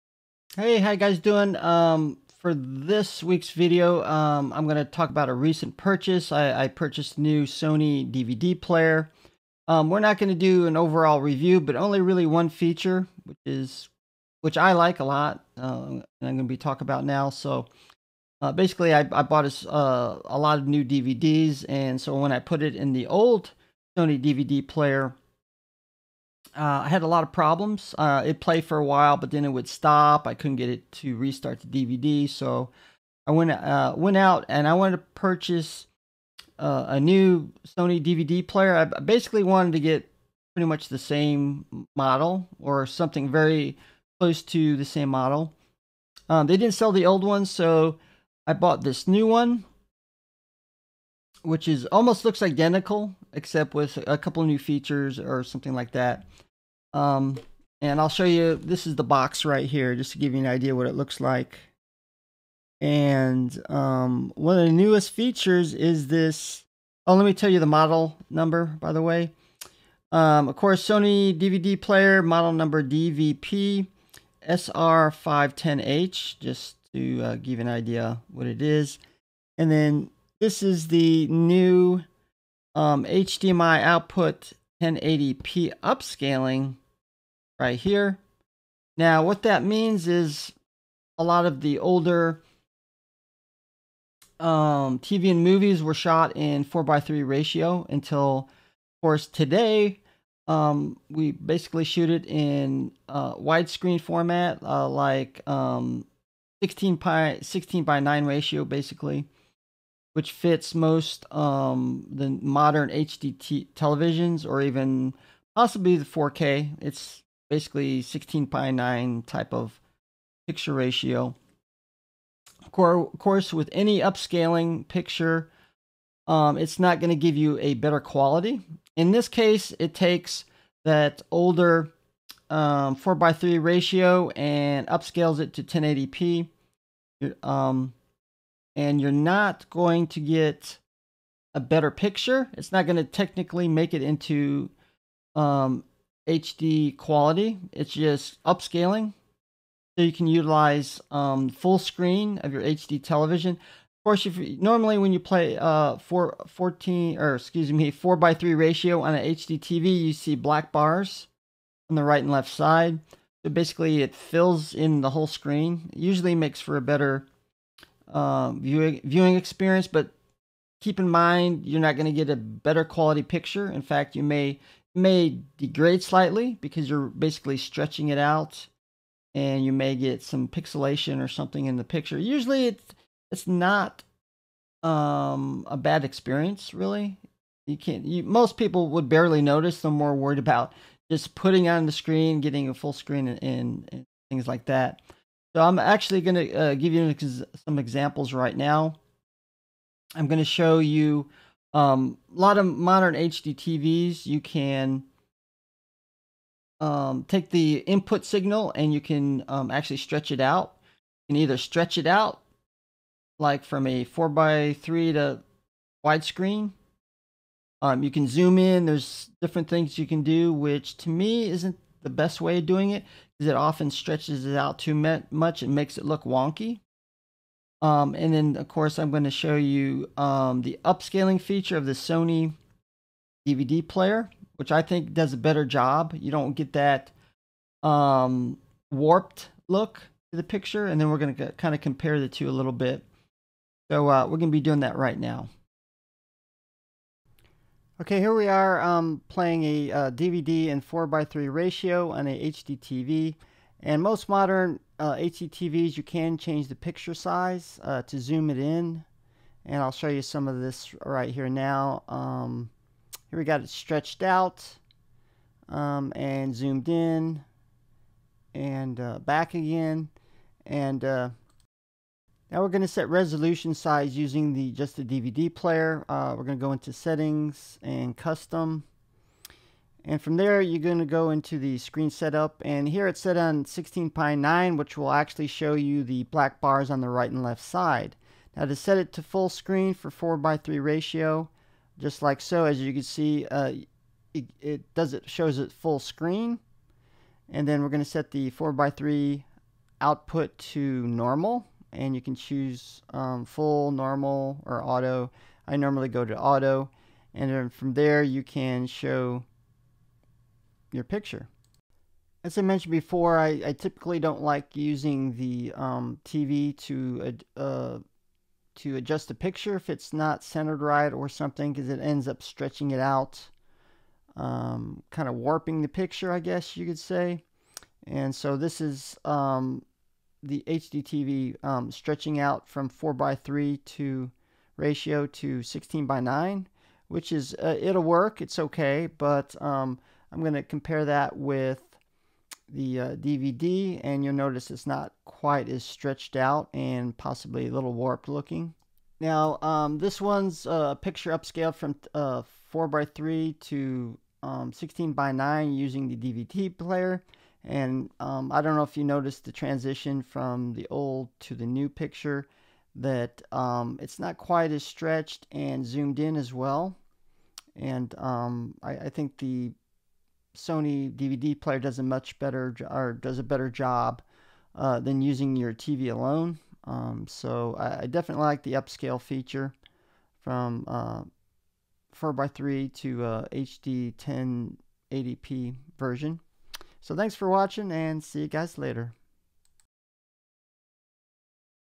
Sony DVD DVP-SR510H
We take a look at the 1080p upscaling feature to watch your old TV show and movies from 4:3 Ratio to full screen 16:9.